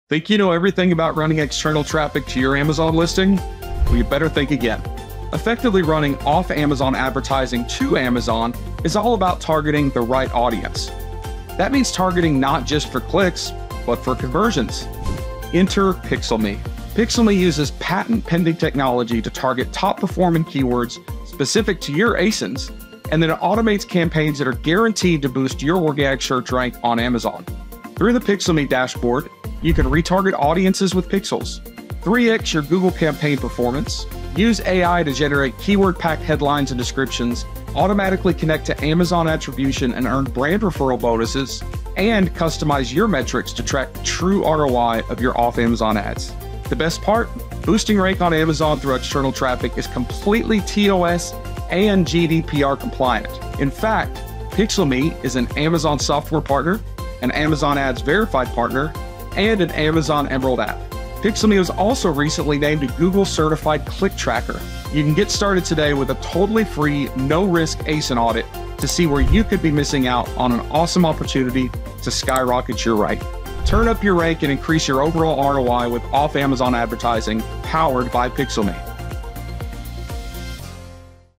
Boost rank and ROAS with external traffic
Boost your rank and ROAS with PixelMe!

PixelMe is the industry-leading off-Amazon advertising software for driving external traffic to Amazon, earning Amazon Attirbution bonuses, and retargeting audiences across platforms to increase ROAS.